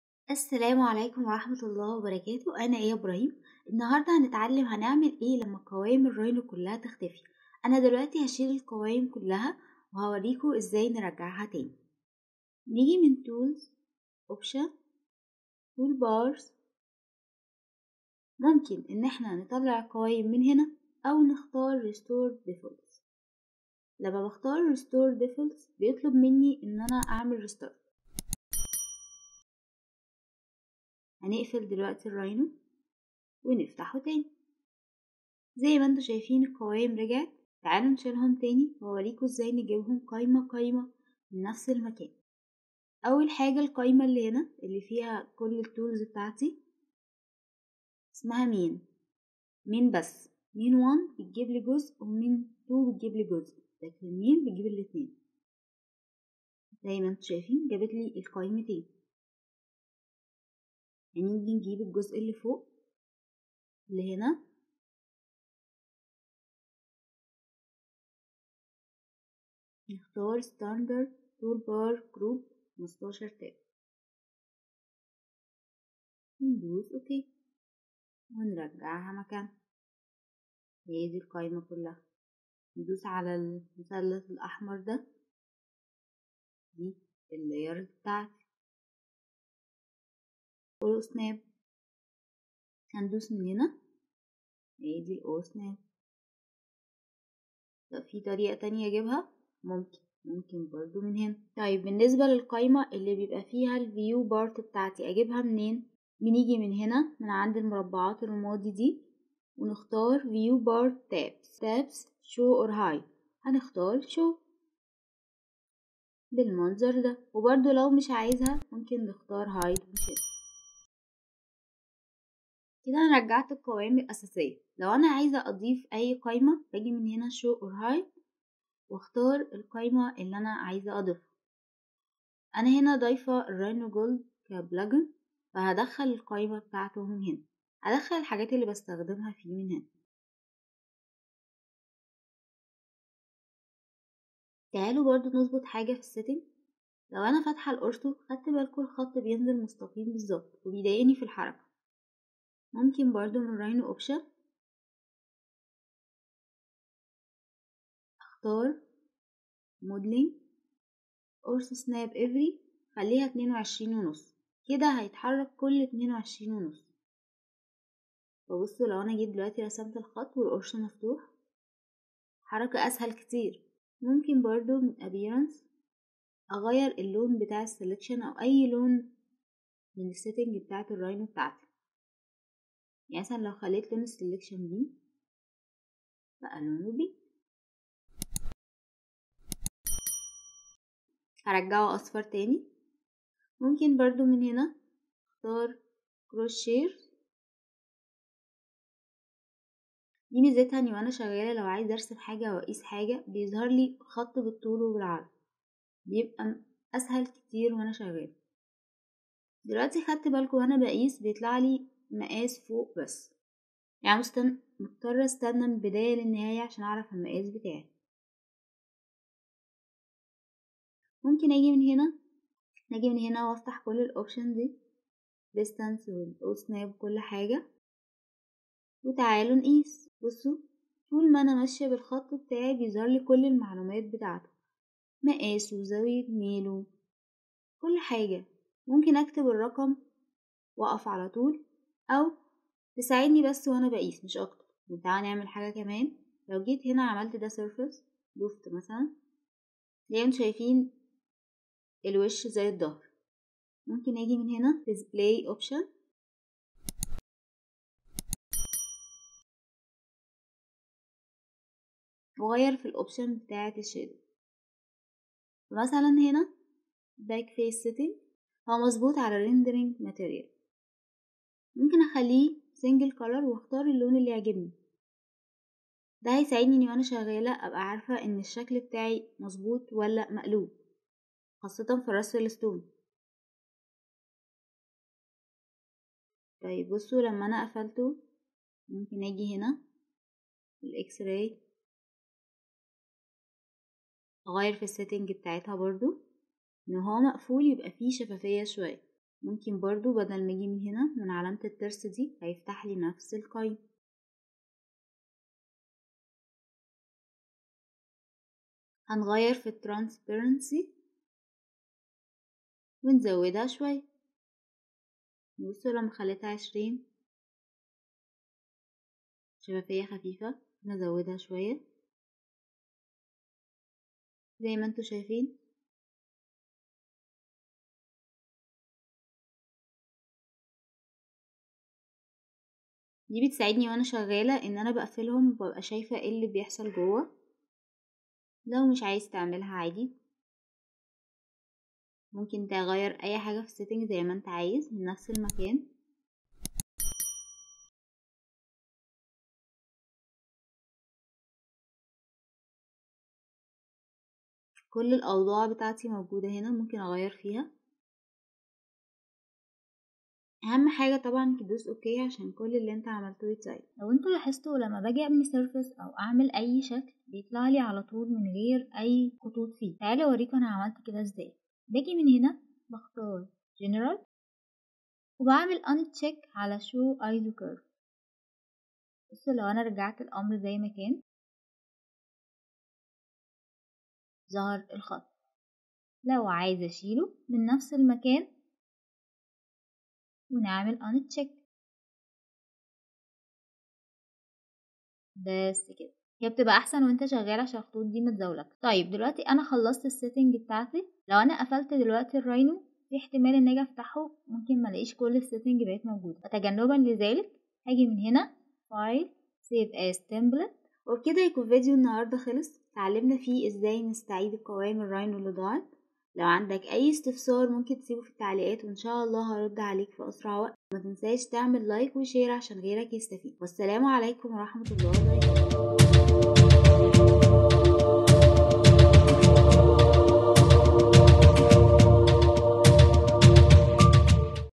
How to Rest Rhino toolbars & Setting up professional interface for Rhino 6
In this video I will teach you two ways to Rest Rhino toolbars and change setting of Ortho, color surface. els

💫for rhino course

الكورس مخفض في هذه اللينك🔥
🔥there is a discount on this link

🔥The survey form tell us what you like to learn about jewelry design 👇

💎If you don't like it, tell us why 😁
💎If you have any suggestions or explanations, you need to leave them in the comments